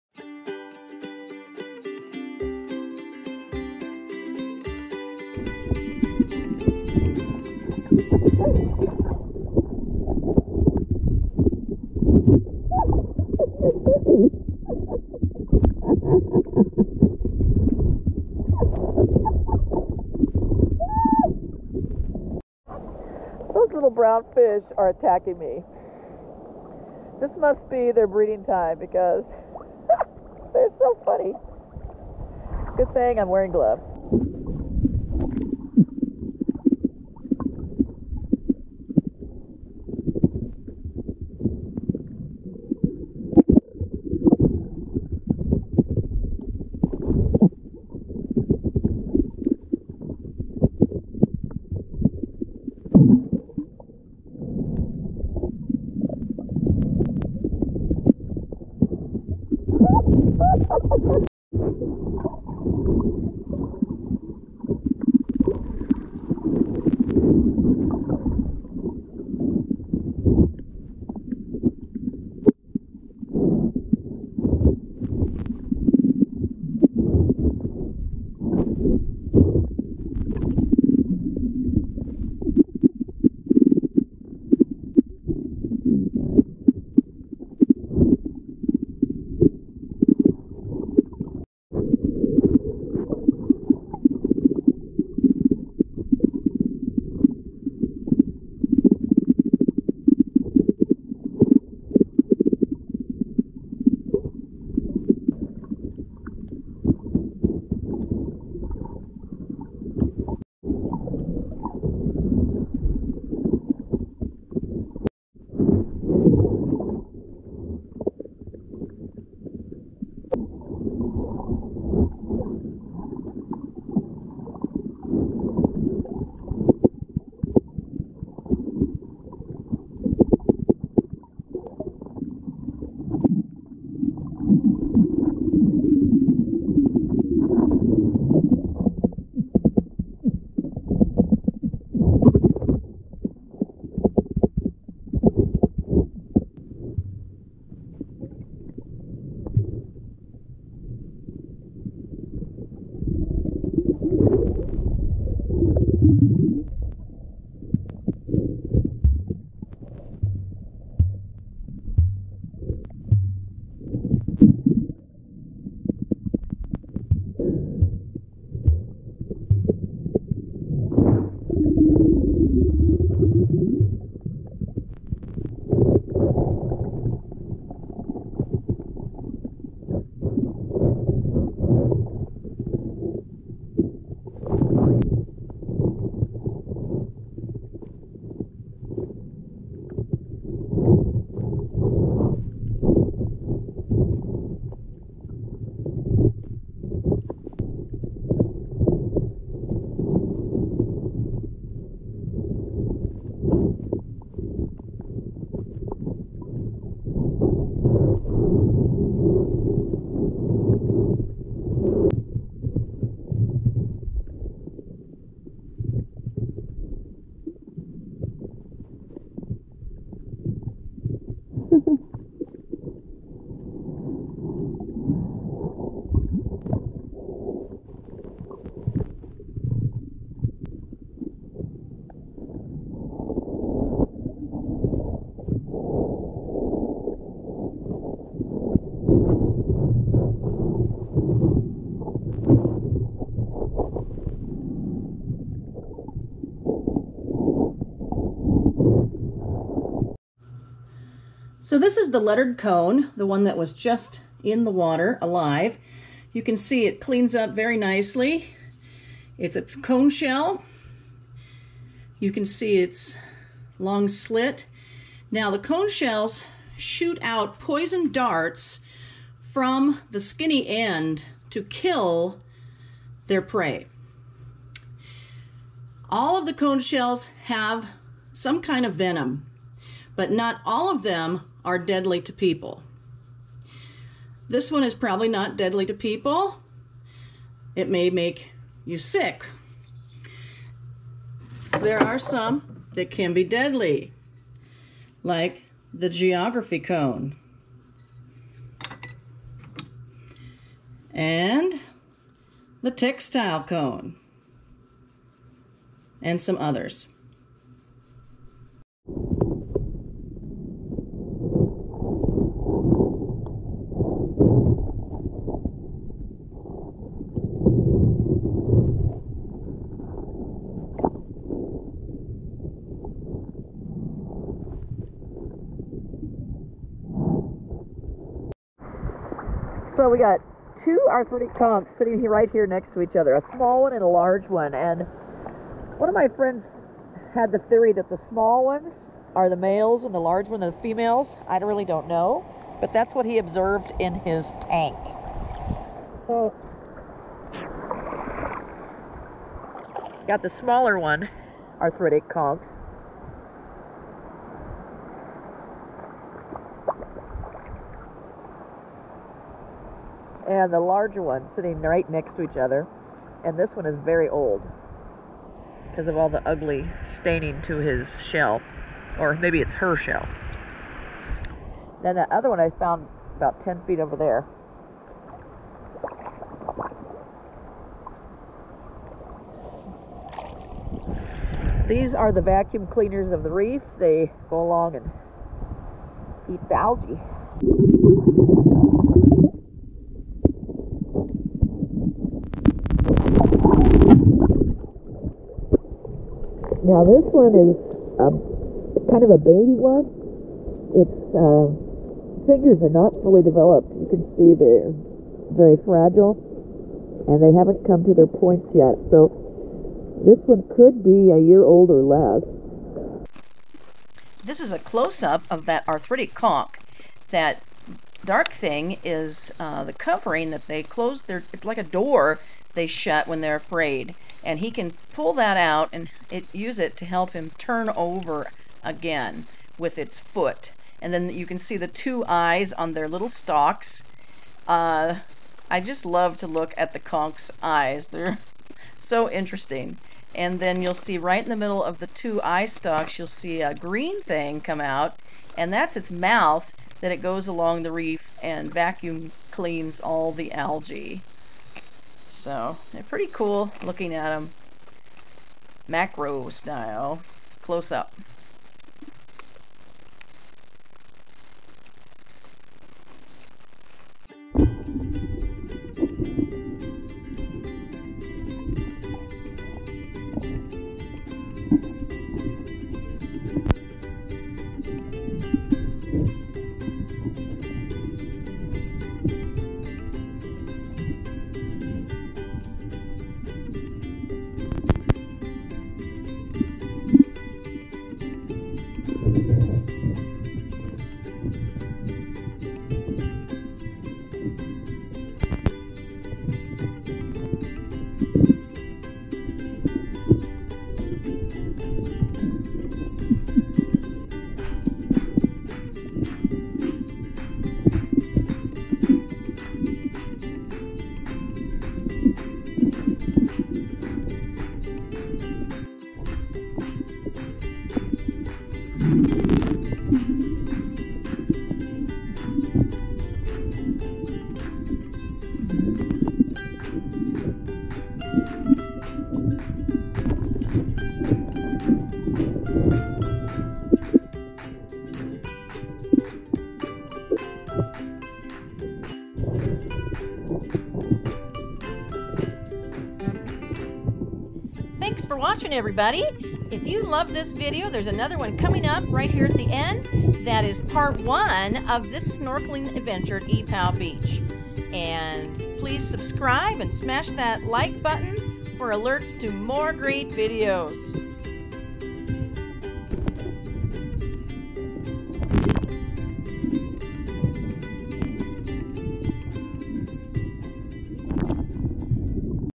Snorkeling Adventure (Watch Out for Those Fish!)
This video is part 2 of snorkeling at Ypao Beach at Tumon Bay, Guam. There are a lot of cool creatures including attacking fish, shells, and other neat fish.